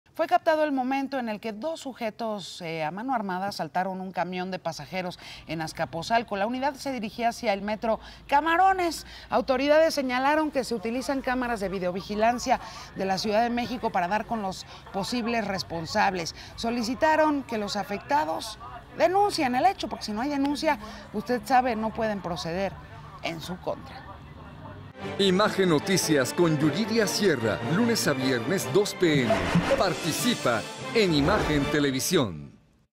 Video de asalto a camión de pasajeros en Azcapotzalco | Noticias con Yuriria Sierra
Una cámara de seguridad captó asalto a camión de pasajeros en Azcapotzalco que se dirigía a Metro Camarones.